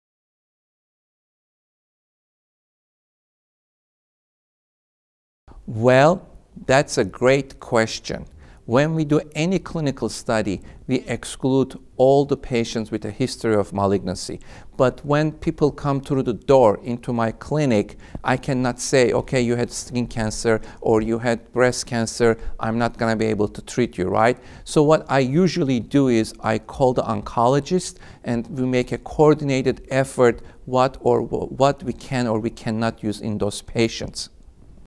Leon Kircik, MD - Can you prescribe dupilumab to patients with a history of malignancy?
ID: 01675